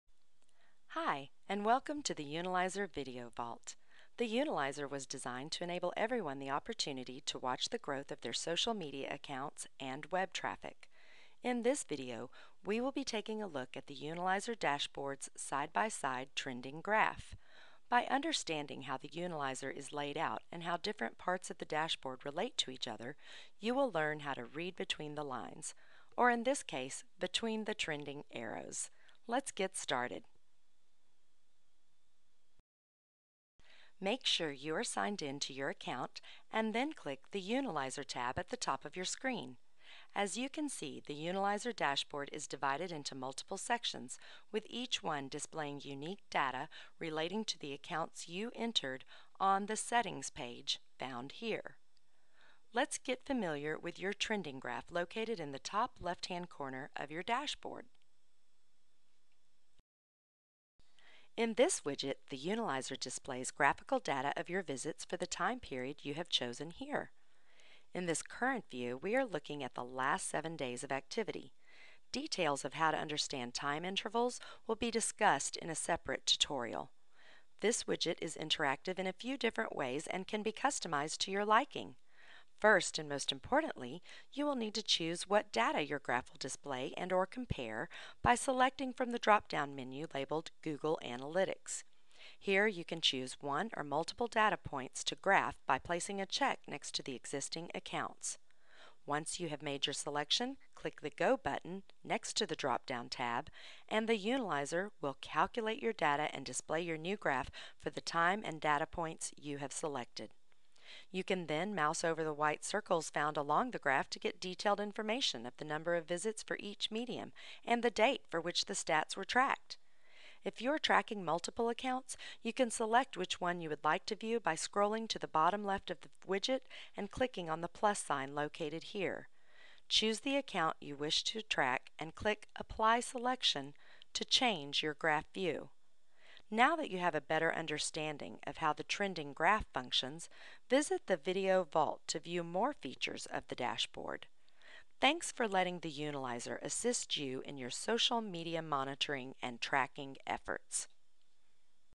Tutorial Video - Understanding your Unilyzer Trending Line Graph WIdget
The Unilyzer was designed to enable everyone the opportunity to watch the growth of their Social Media accounts and web traffic.  In this video, we will be taking a look at the Unilyzer dashboard and some of its basic features.  By understanding how the tool is laid out and how different parts of the dashboard relate to each other, you will learn how to read between the lines, or in this case between the trending arrows.  Lets get started.
Sections:  The Graph  (Trend Line Graph)
Make sure you are signed in to your account and then click the Unilyzer tab at the top of your screen.  As you can see, the Unilyzer dashboard is divided in to multiple sections, with each one displaying unique data relating to the accounts you entered on the Settings page found here (mouse over settings tab).  Lets start at the top left hand corner and move clockwise through the dashboard.
In this widget, the Unilyzer displays graphical data of your visits for the time period you have chosen here (mouse over the time intervals).  In this current view, we are looking at the last 7 days of activity.  Details of how to understand time intervals will be discussed in a separate tutorial.  This widget is interactive in a few different ways and can be customized to your liking.  First and most importantly, you will need to choose what data your graph will display and/or compare by selecting from the dropdown menu labeled Google Analytics.  Here you can choose one or multiple data points to graph by placing a check next to the existing accounts.  Once you have made your selection, click the Go button next to the dropdown tab and the Unilyzer will calculate your data and display your new graph for the time and data points you have selected.  You can then mouse over the white circles found along the graph to get detailed information of the number of visits for each medium and the date for which the stats were tracked.  If you are tracking multiple accounts, you can select which one you would like to view by scrolling to the bottom left of the widget and clicking on the plus sign located here.  Choose the account you wish to track and click apply selection to change your graph view.